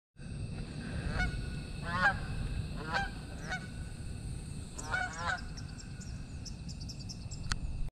Geese Flying
Watch some Canada Geese flying in a perfect sky.